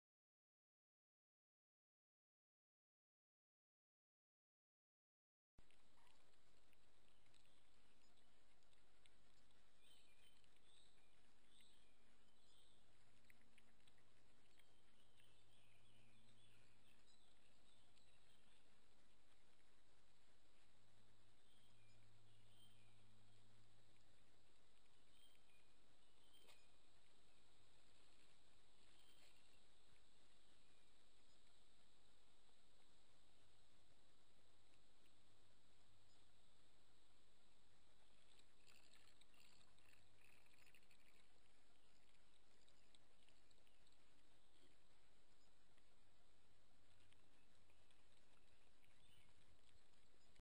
Badger carrying cub by scruff of neck back to sett!!
Badger clips taken at my local sett in Devon.

Michael Leigh-Mallory.

All clips best viewed in HD.